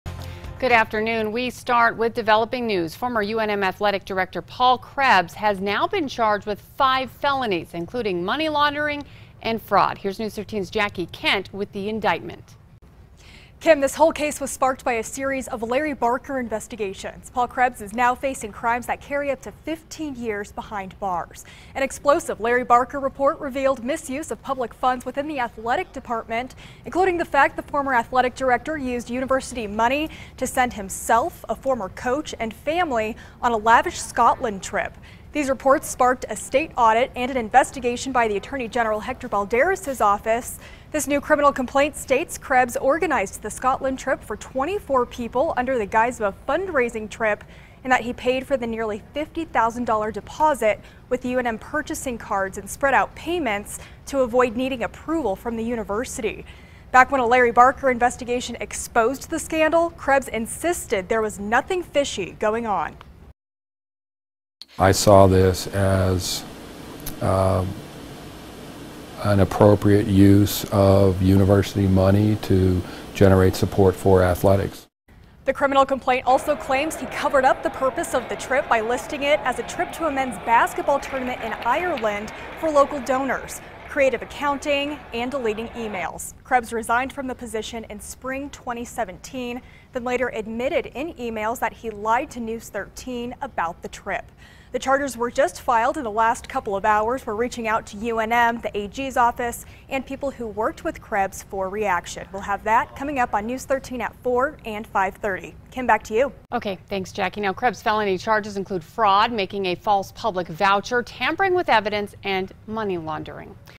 Former UNM Athletic Director Paul Krebs charged with fraud, money laundering
Former UNM Athletic Director Paul Krebs charged with fraud, money laundering - Source